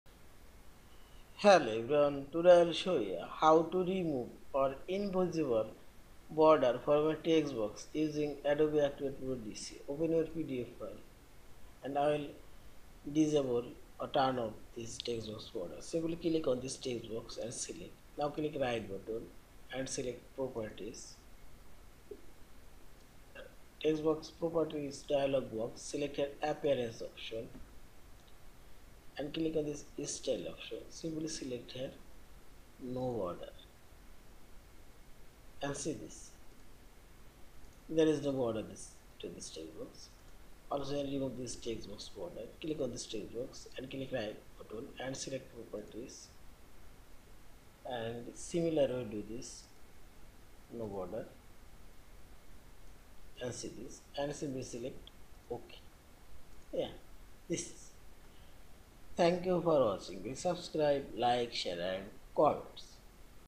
How to remove the border from a comment text box in a PDF file using Adobe Acrobat Pro DC
Hello everyone, Today I will show you. How to remove the border from a comment text box in a PDF file using Adobe Acrobat Pro DC.